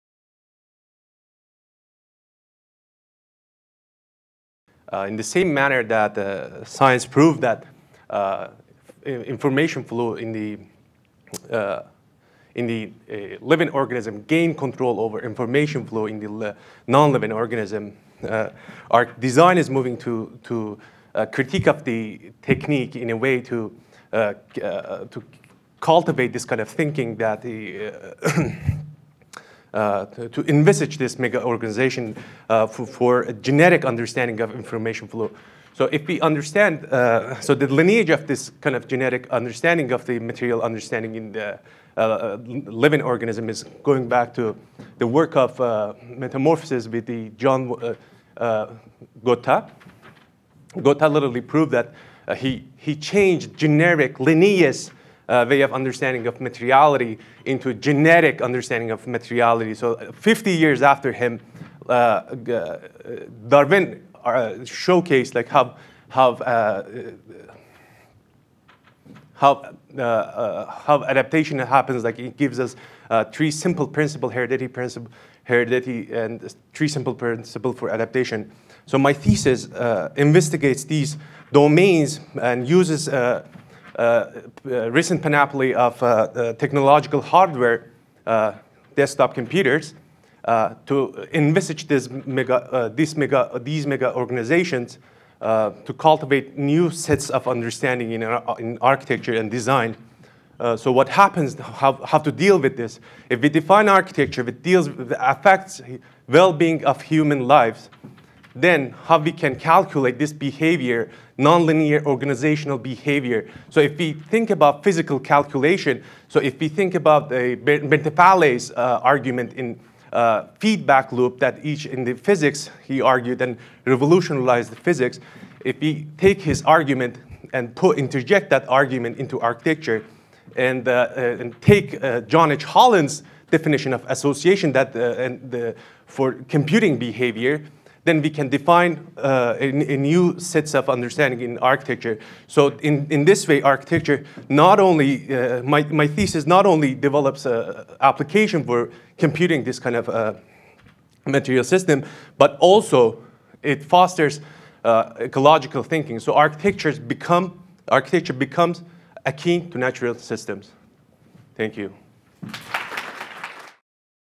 2016 Ryerson 3MT Participant: Babak Teimoori
“Material computation for new equilibrium in architecture (cultivating ecological thinking)”

- Babak Teimoori, Architecture